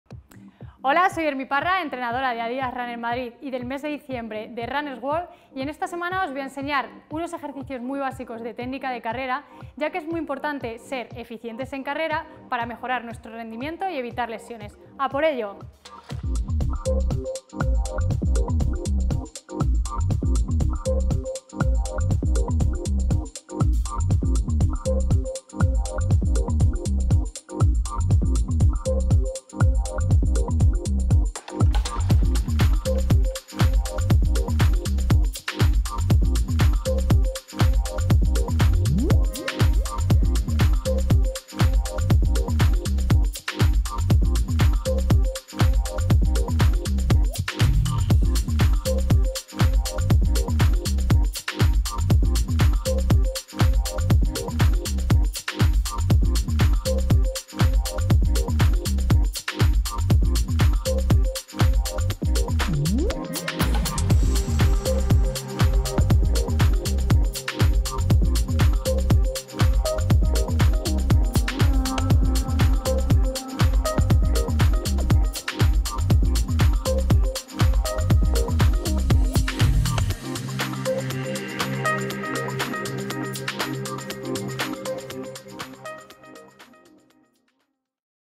Semana 4 con Hermi Parra: Rutina de ejercicios básicos de técnica de carrera |Runner's World España
Semana 4 con Herminia Parra, Hermi en el mundo del deporte y atleta internacional y entrenadora de  Adidas Runner's Madrid, es la 'Entrenadora del Mes' de diciembre de Runner's World. Estos ejercicios de  técnica de carrera te prepararán para mejorar la postura, el rendimiento y prevenir lesiones en carrera.

Canal en el que encontrarás vídeos sobre el mundo del running. Vídeos para corredores con información sobre entrenamientos, salud y nutrición...